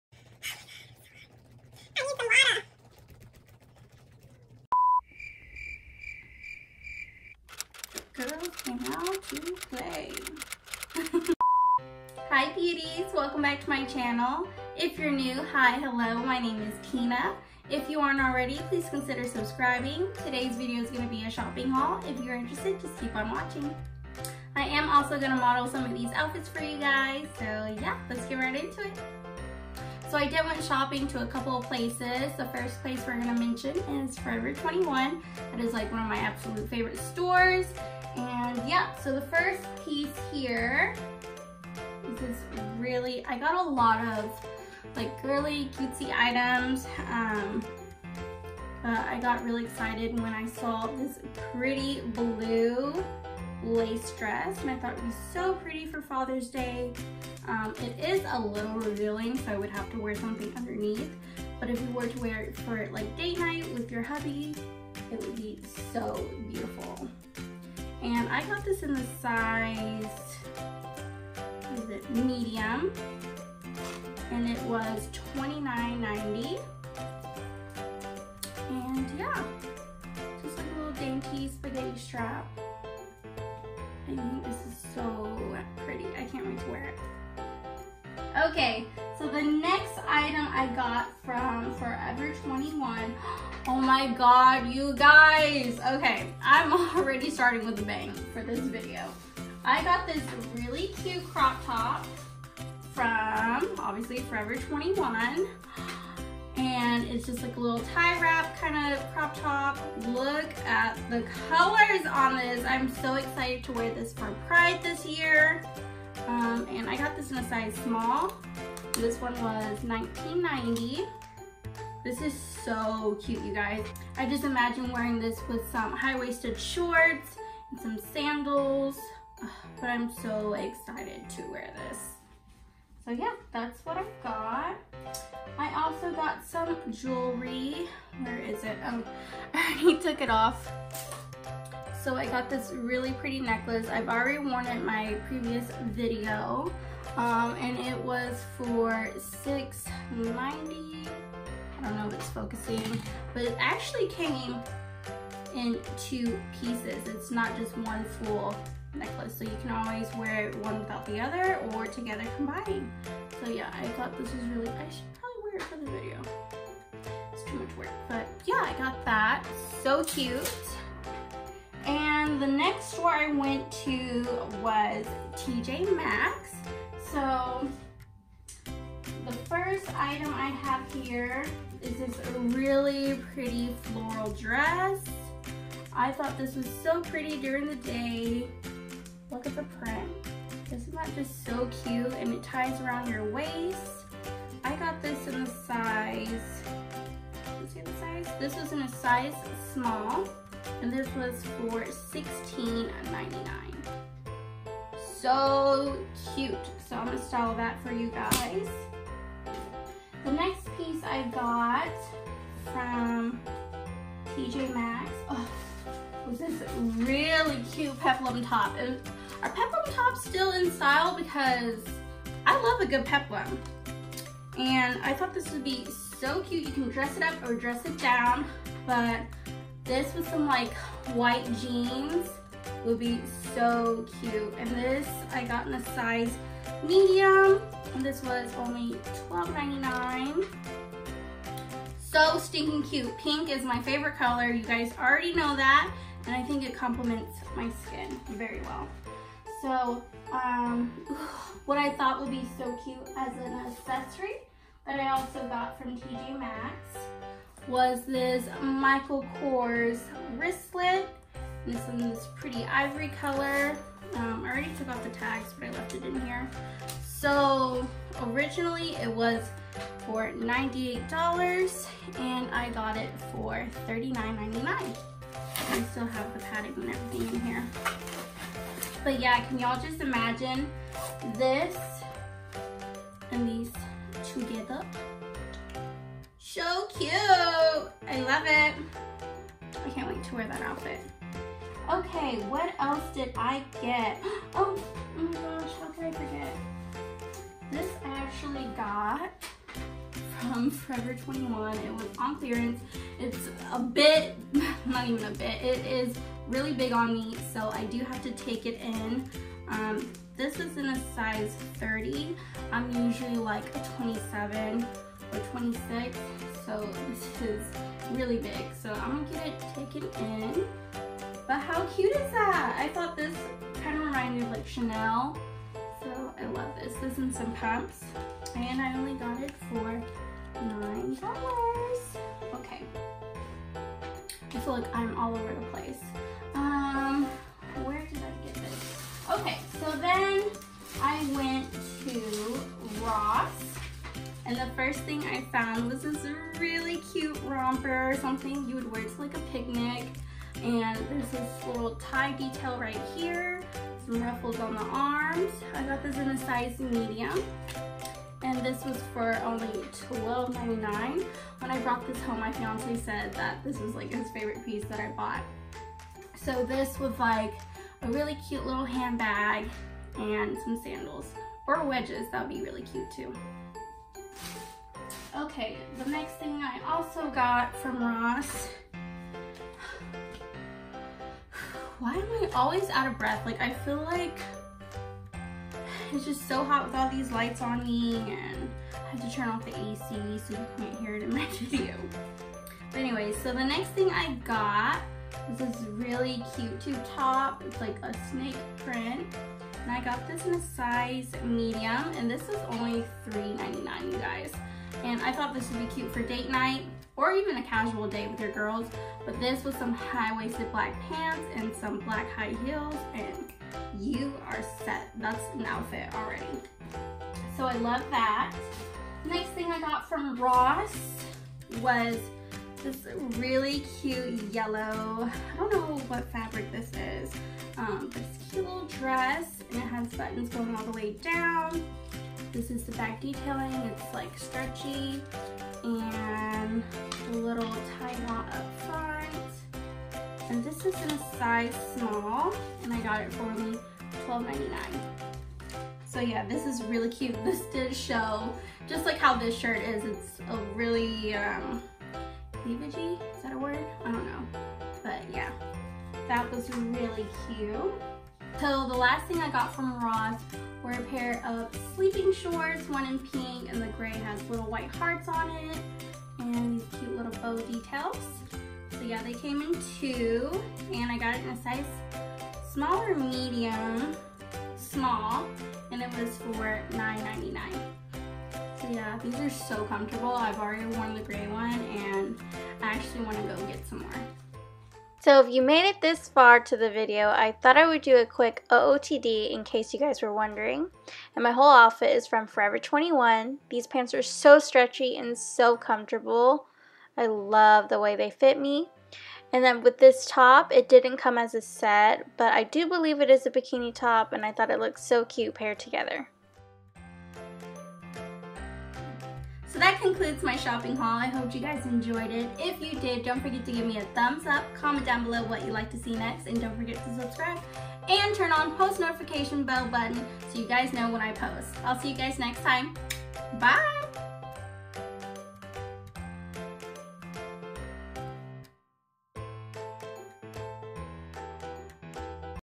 Shopping Haul | Try On Video
I'm back with another fun video for you guys. I went shopping couple weeks ago and I thought you guys would like to see a little collective haul that I've been dying to share. I had so much fun playing dress up with you and I hope you guys pick up some of these pretty little pieces for yourself to wear this summer. :D